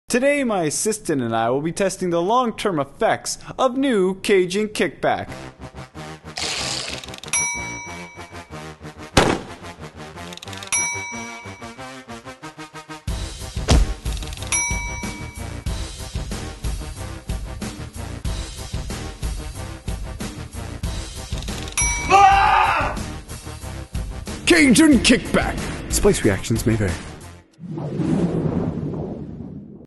Cajun Kickback - Delayed Reactions
A Scientist and his Assistanttest the long term affects of new Dorito's Cajun Kickback.